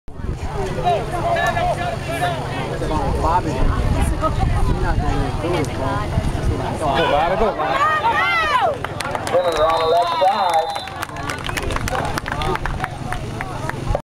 Bobby Simmons #22 Assumption Mustangs Football
Bobby Simmons #22 runs the fiitball